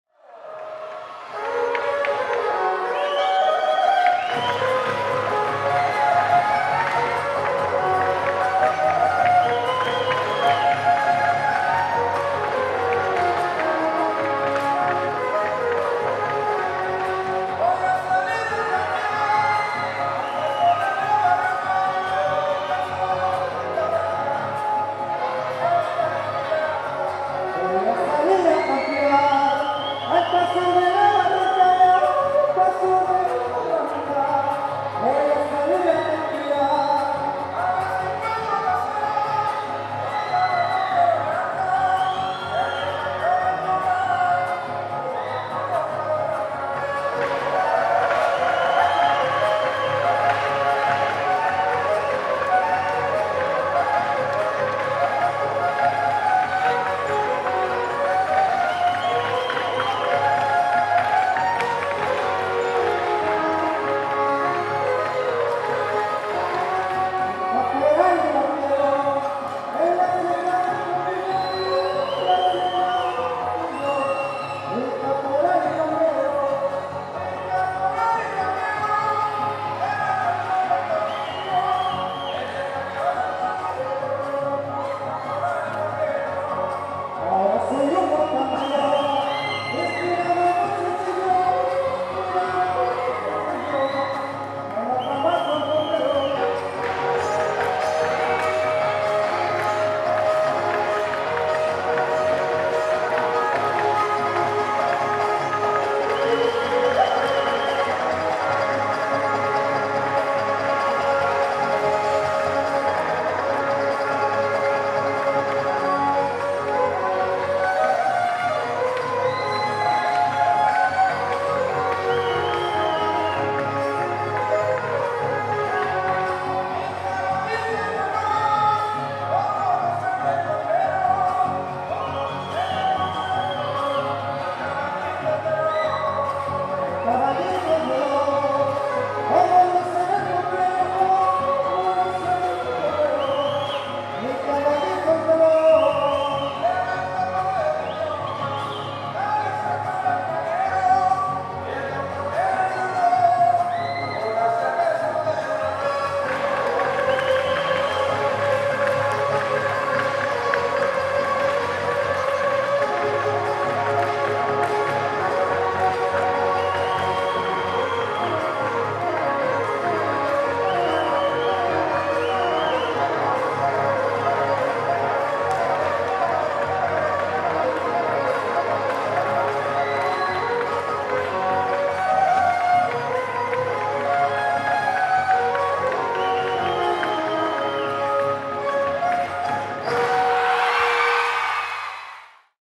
Trío Valle Dorado - Huapango infantil estilo veracruzano en Tuxpan 2016
un poco de lo que se vivió en el concurso Nacional de Huapango Tuxpan 2016. El Trío  Valle Dorado amenizando el concurso.  Contrataciones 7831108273, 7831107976 y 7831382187